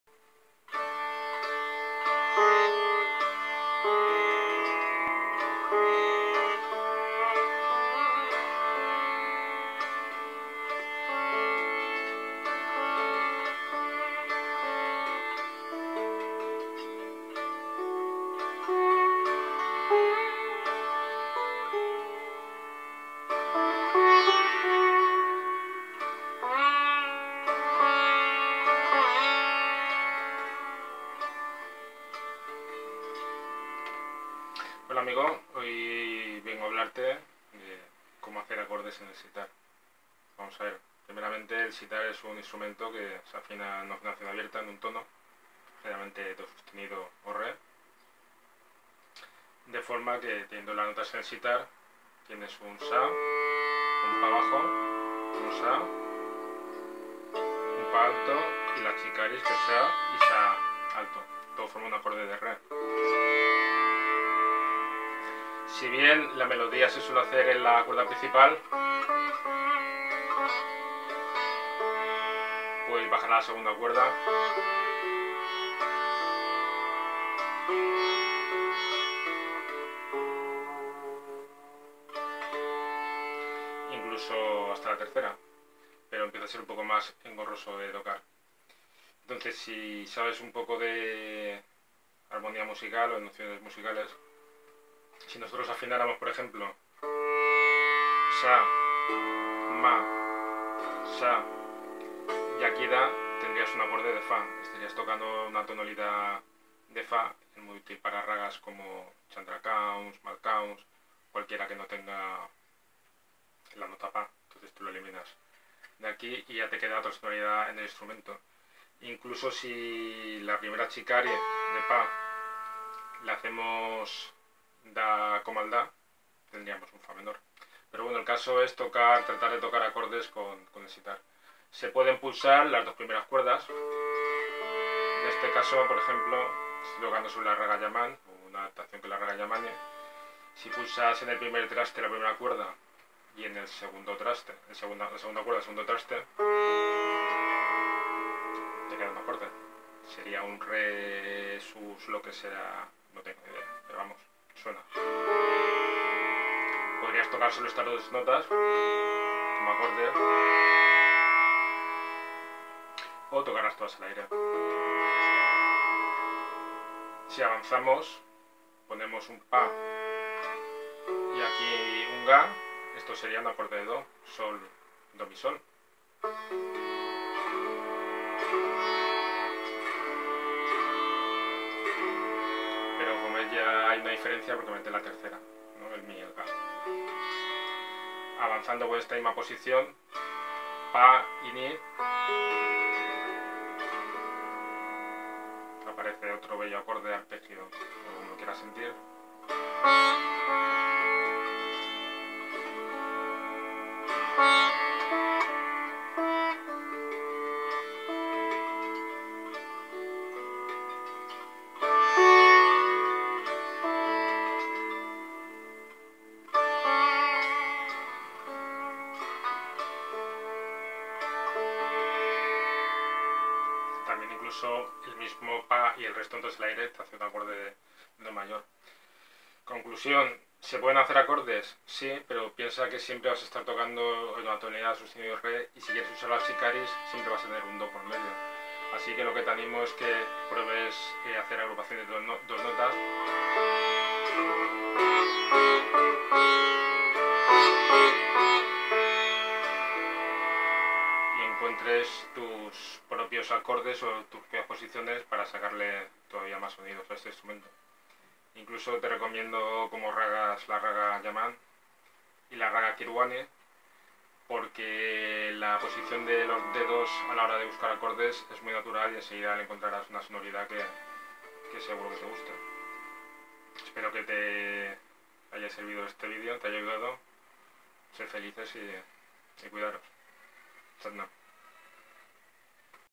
Clases de Sitar Online : Acordes con un sitar
¿Es posible hacer acordes en el sitar?
Aunque este magnífico instrumento suele estar afinado en una tonalidad abierta de C# o D, y parte de las cuerdas principales se tocan al aire, es posible pulsar mas de una cuerda a la vez, generando increíbles combinaciones.

Si sabes un poco de armonía musical y generación de acordes todavía te será más fácil...

Cuida tu mente y que no pare la música¡